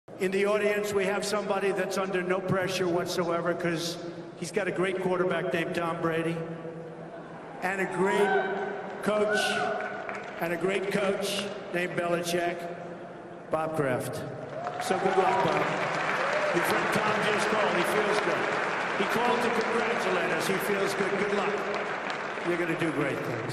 Trump Thanks Robert Kraft At Pre-Inauguration Dinner
President-Elect Donald Trump wished Kraft luck and said Tom Brady called to congratulate him.